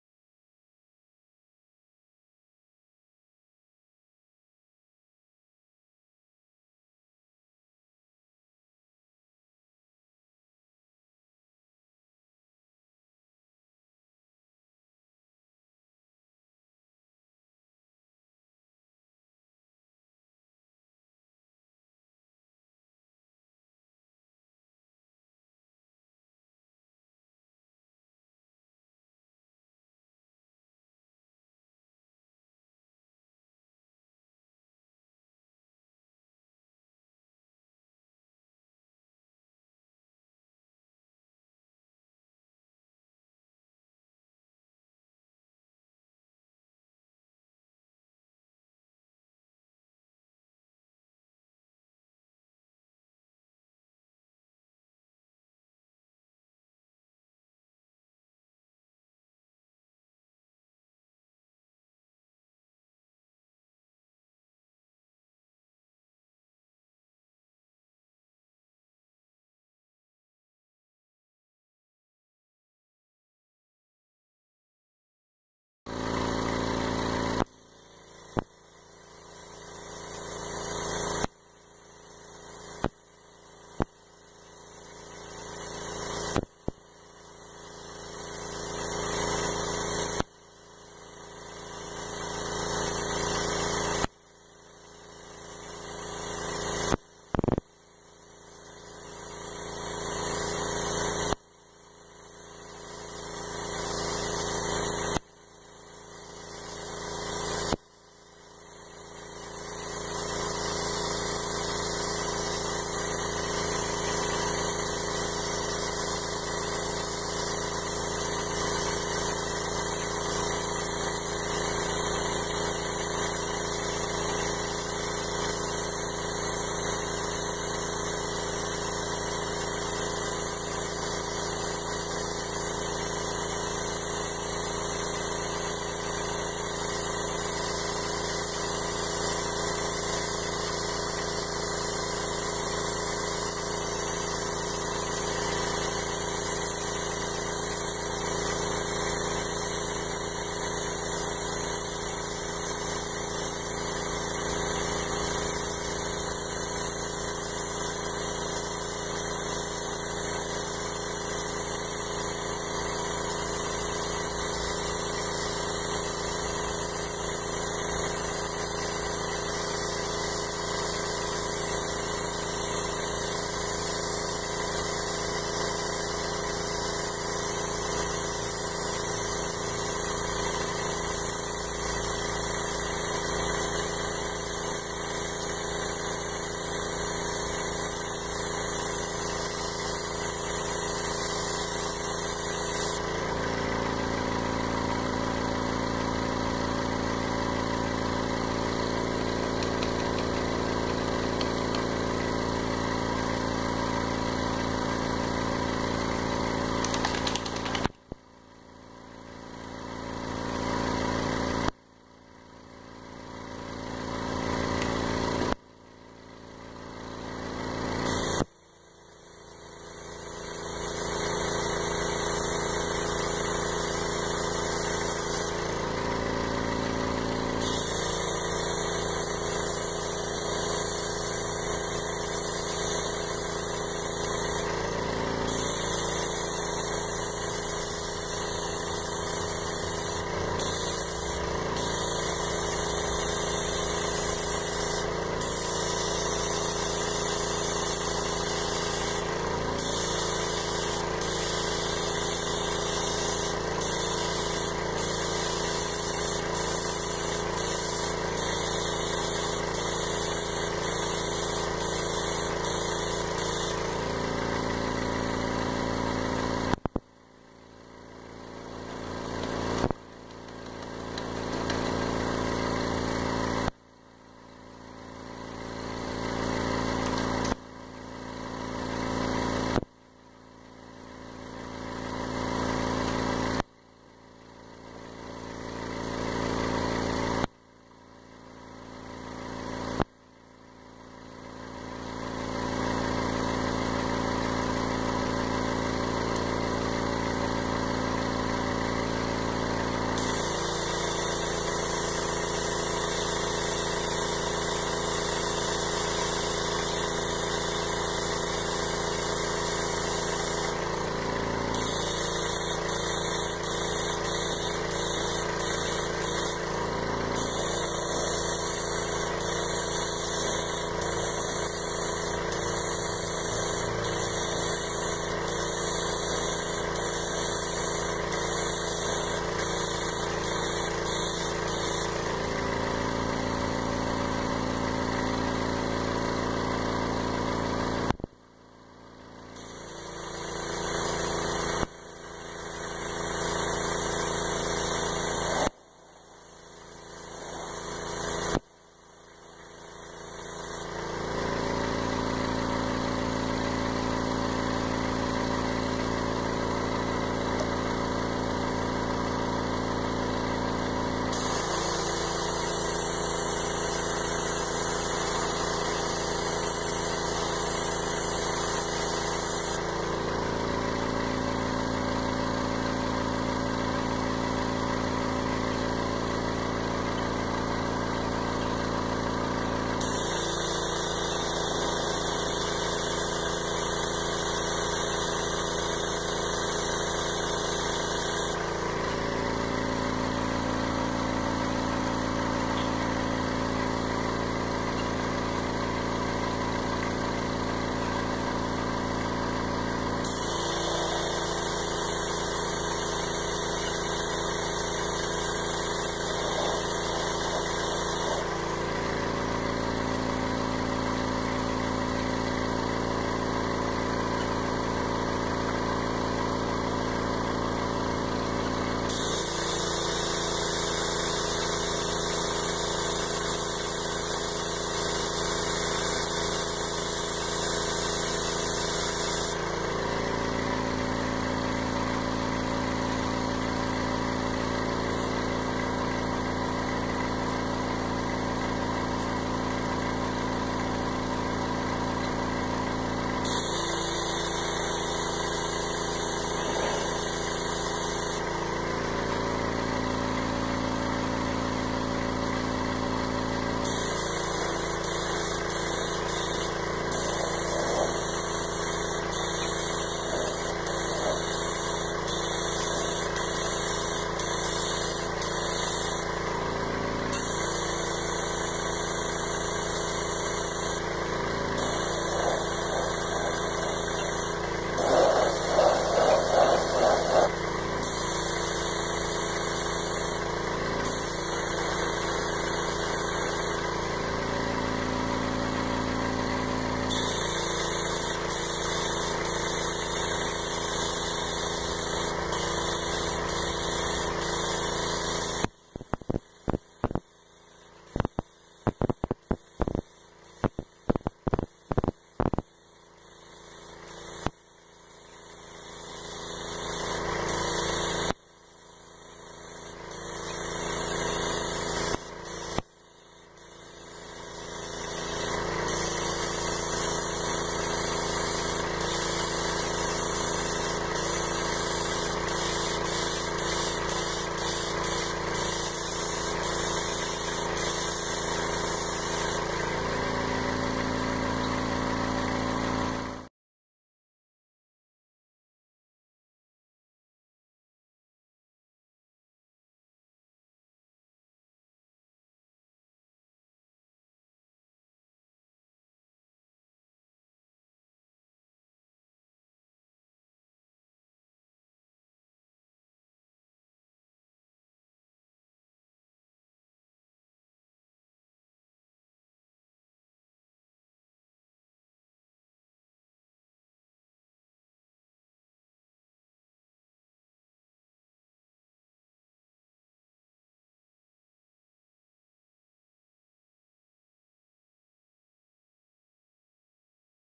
How to Apply a Snakeskin Graphic to a Guitar
If you've ever wondered how to airbrush a snakeskin graphic on a guitar (or anything), this video walks you through it.